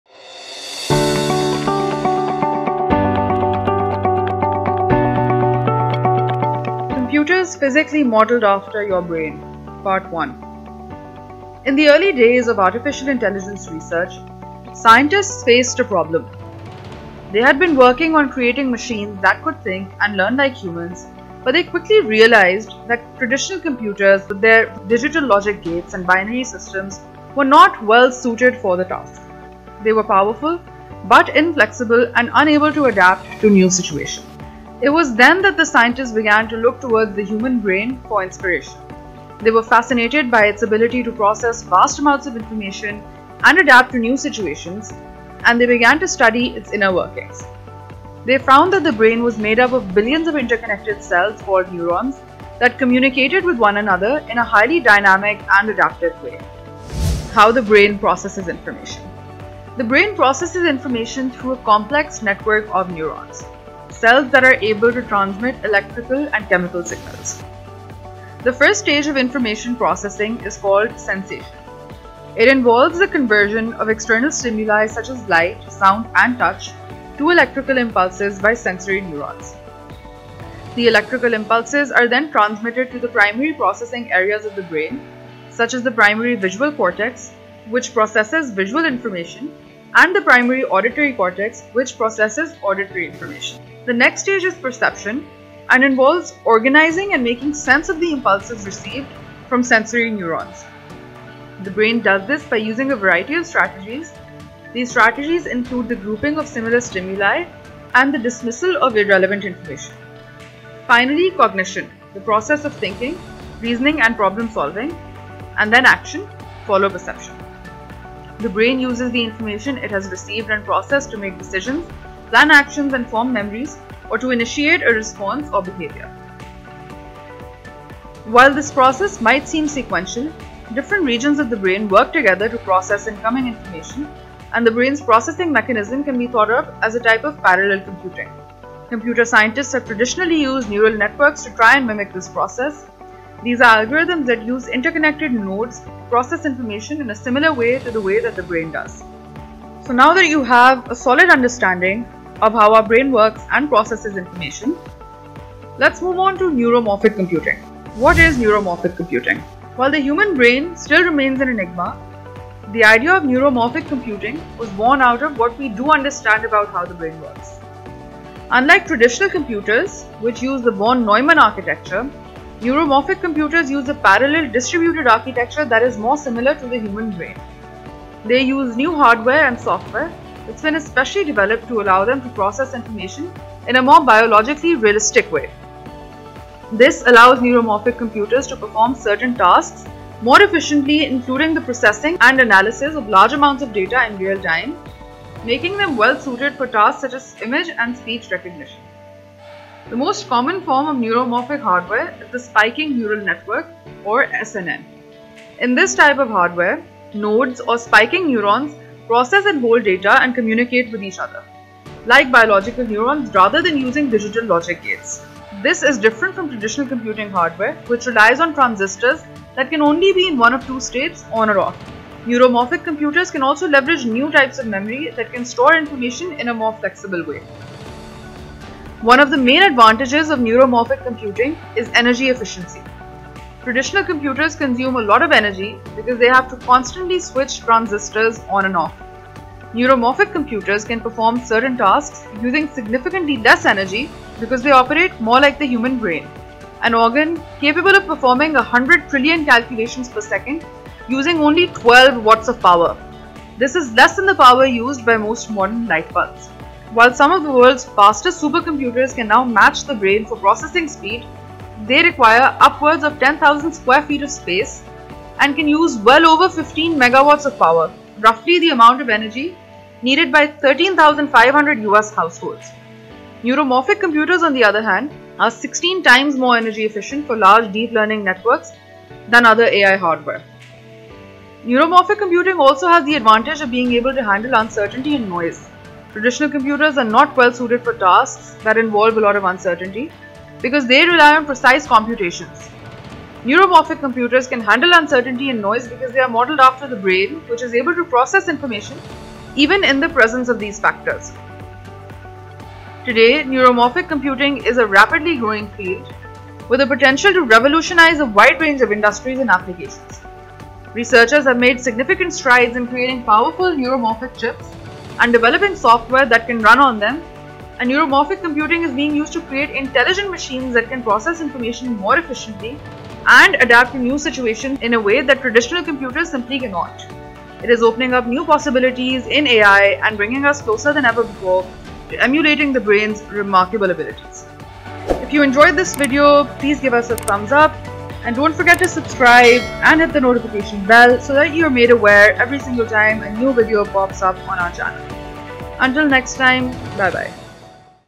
What Is Neuromorphic Computing?
While the human brain still remains an enigma, the idea of neuromorphic computing was born out of what we understand about how the brain works. Unlike traditional computers, which use Von Neumann architecture, neuromorphic computers use a parallel, distributed architecture that is more similar to the human brain. They use new hardware and software that’s been especially developed to allow them to process information in a more biologically realistic way. This allows neuromorphic computers to perform certain tasks more efficiently including the processing and analysis of large amounts of data in real-time, making them well-suited for tasks such as image and speech recognition.